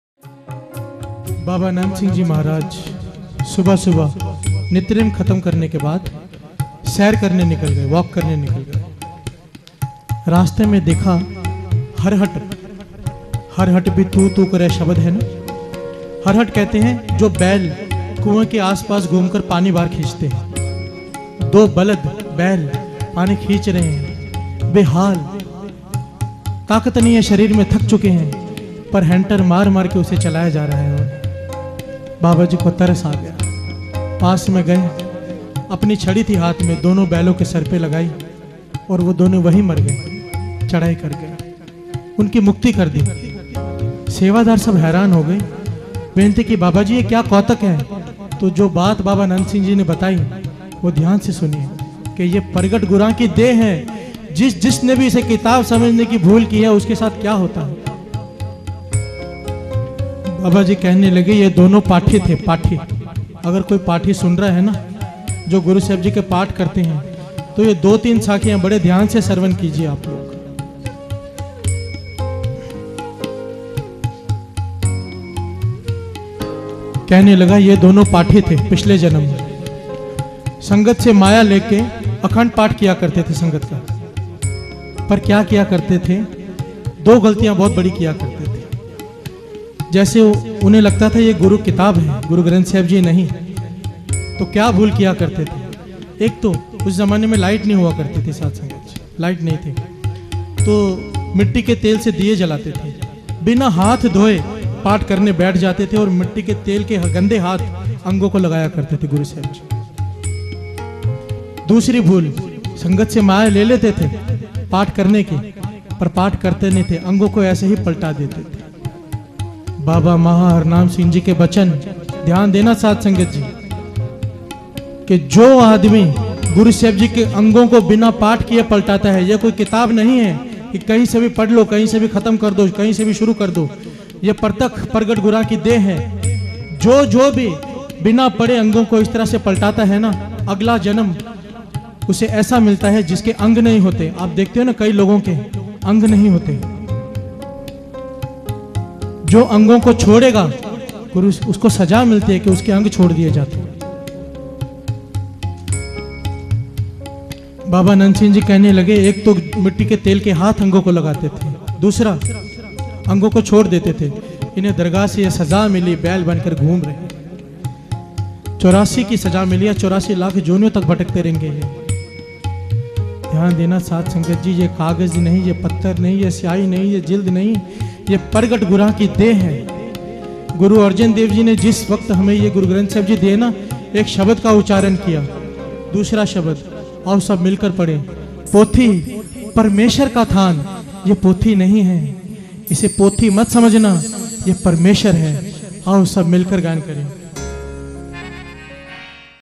Jis Jis Ne Isse Kitab Samajhne Ki Bhool Ki Hai - #8 oldkirtan | Bhai Gurpreet SIngh (Rinku Veerji)
Dhan Guru Gobind Singh Ji
Dhan Baba Deep Singh Ji

Title- Jis Jis Ne Isse Kitab Samajhne Ki Bhool Ki Hai... Uske Sath Kya Hua Hai !
Part- 8
By- Bhaisahab Bhai Gurpreet Singh (Rinku Veerji)
Date- 1st September 2013

If you want to listen full Kirtan .. Play video in Playlist in sequence

Another OldKirtan Playlist Links :-